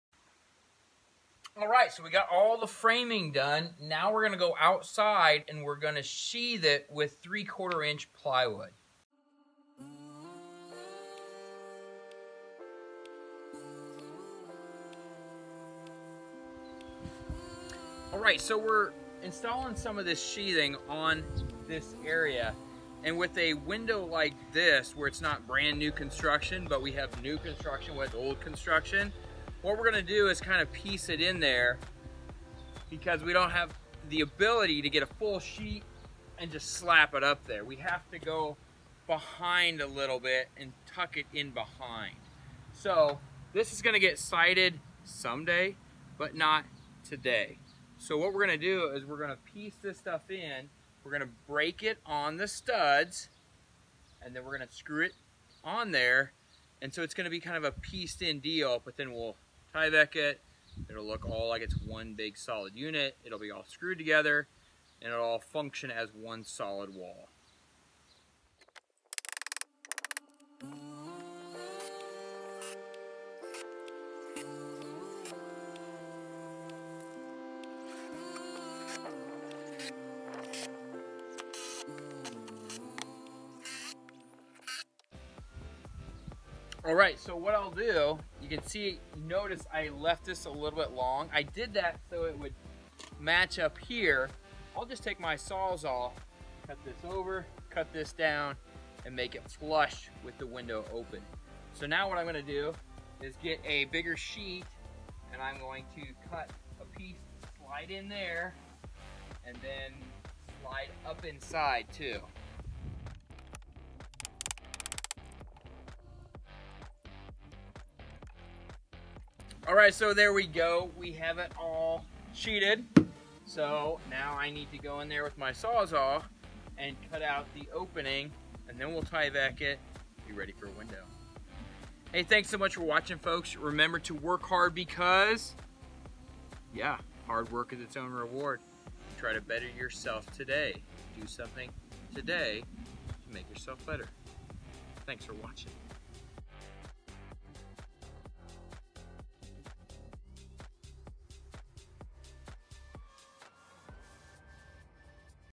How to install sheathing on the outside of a new window opening
In this video, Mr. Kongable shows how to install sheathing on the outside of a window opening that is new construction around old construction.